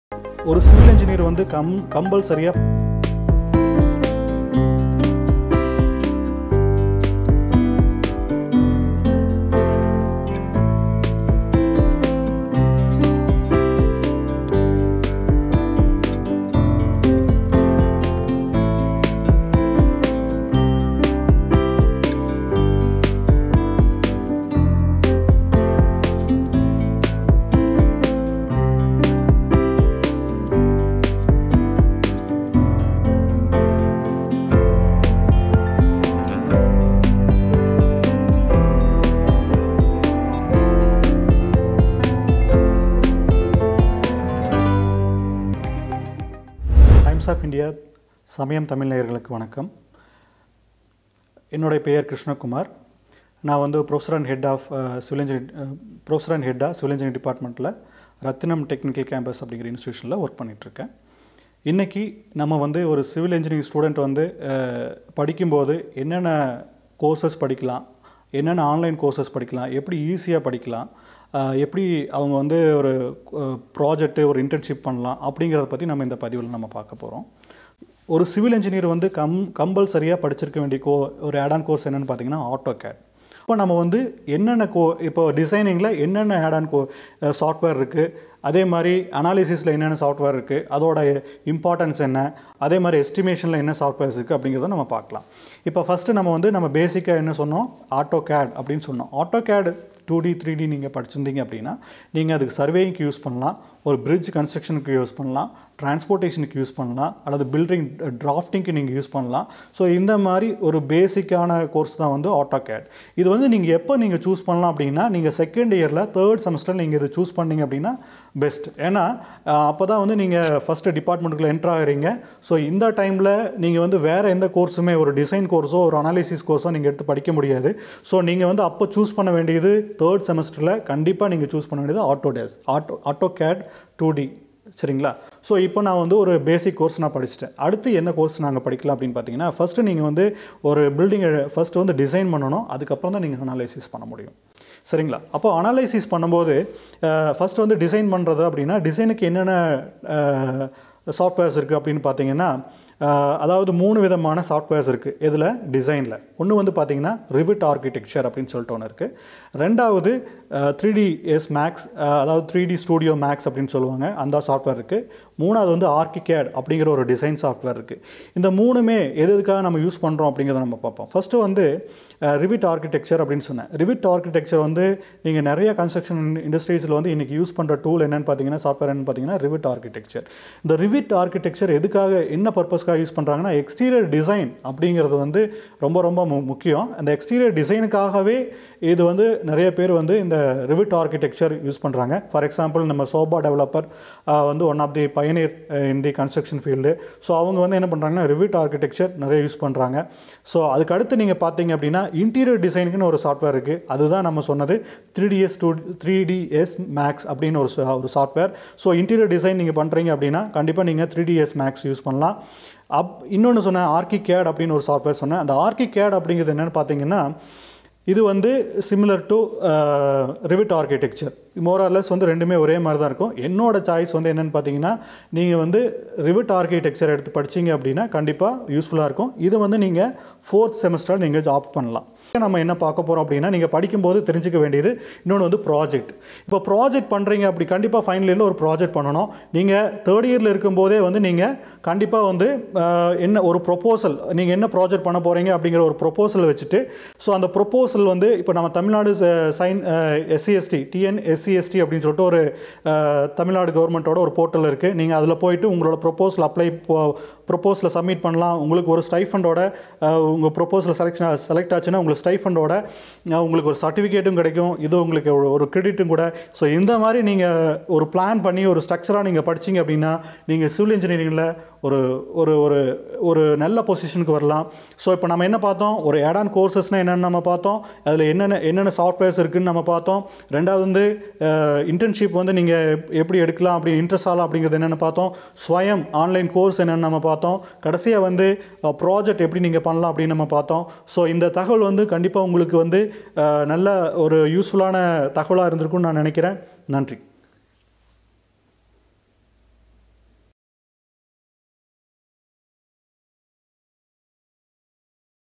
Civil Students Must Learn These Softwares | சிவில் இளைஞர்கள் தெரிந்து கொள்ள வேண்டிய Softwares
சிவில் இளைஞர்கள் படிக்கும் போது தெரிந்து கொள்ள வேண்டிய மென்பொருள் (softwares) என்னென்ன என்பைதை பற்றி தெளிவாக விளக்குகிறார் சிவில் இன்ஜினியரிங் துறைத்தலைவர் மற்றும் இனண பேராசிரியர் திரு. கிருஷ்ணக்குமார் அவர்கள்